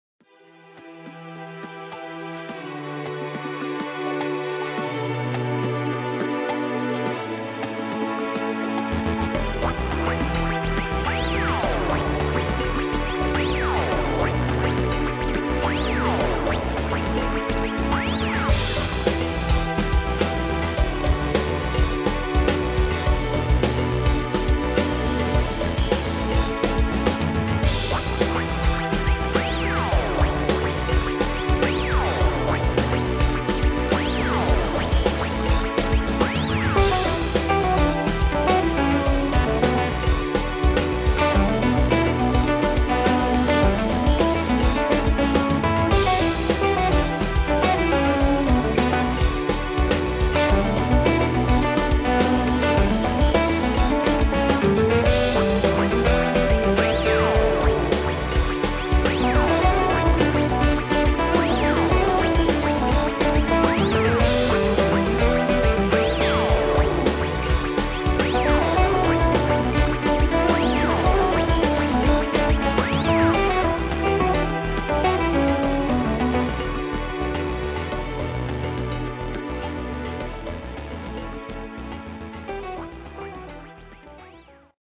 Look at the size of this thing mug
A video Description of the giant size "Look At The Size of This Thing space themed ceramic mug. About a third bigger than the standard coffee mug, it has internal and external decal on the body.